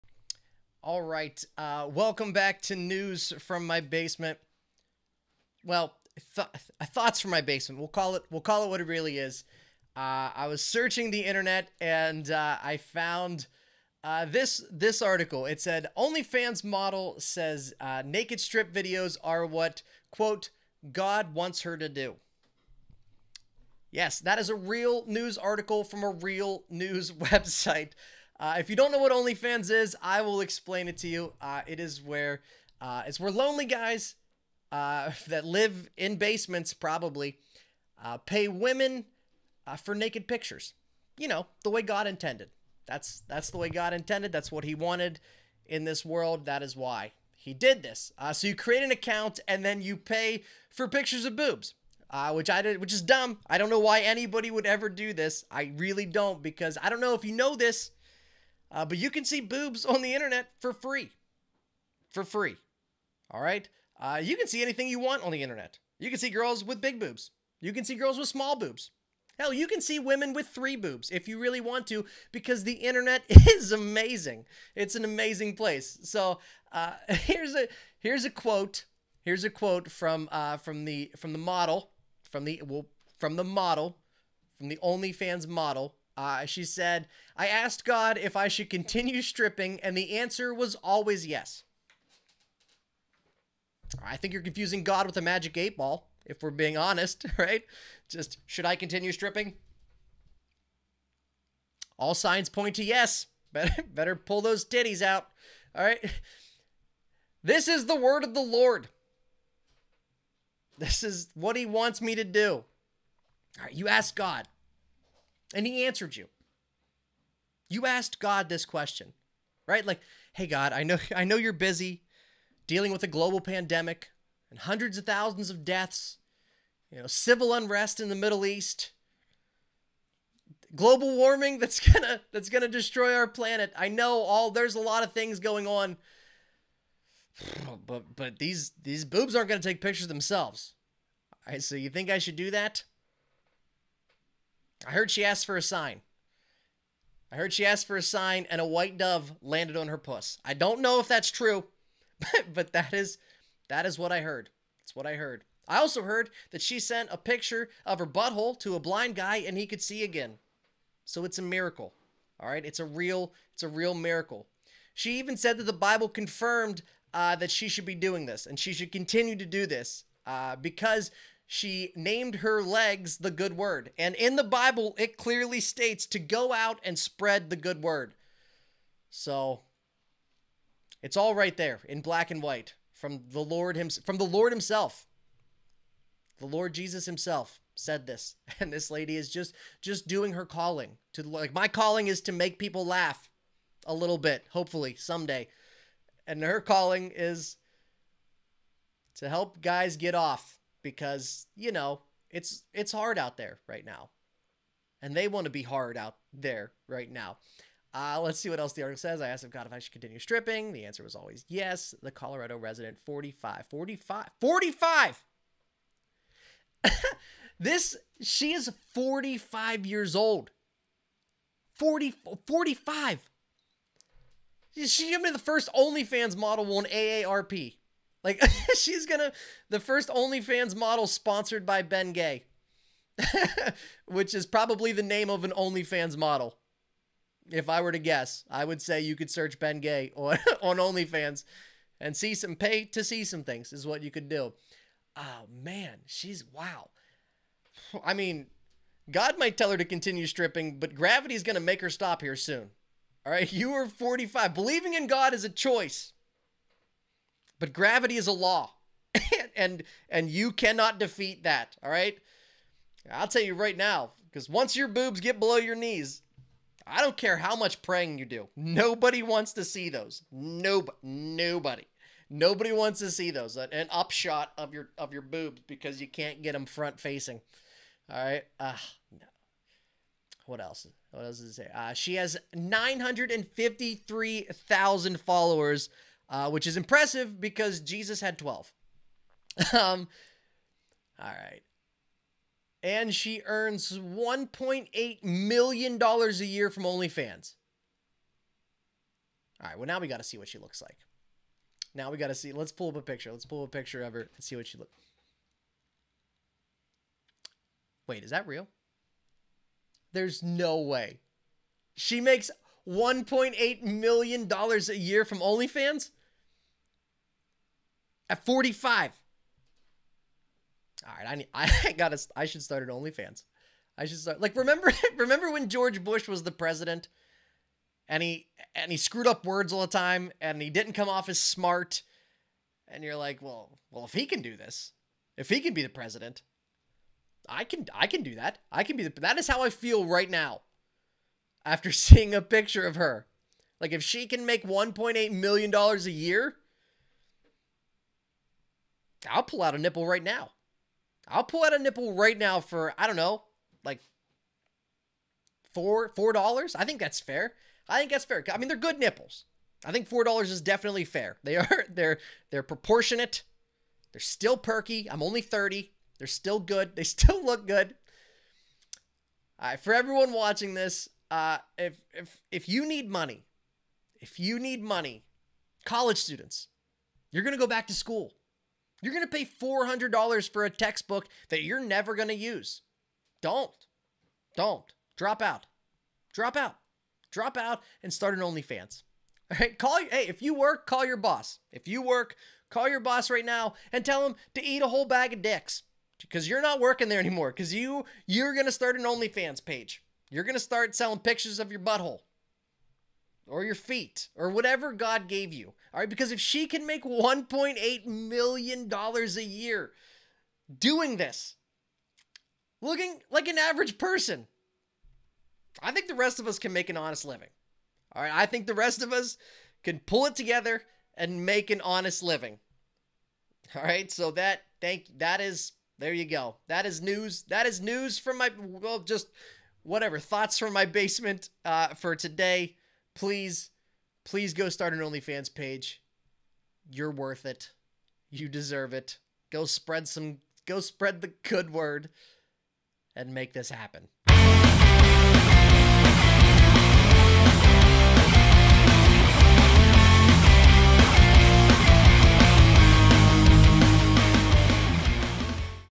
God wanted me to start an OnlyFans | Thoughts From My Basement
A 45 year old model on OnlyFans said she spoke to God and he wants her to strip for her subscribers on OnlyFans... Oh and she's making $1.8 MILLION a year doing it. Yeah, sign me up. I'm in for that. Who wants some feet pics?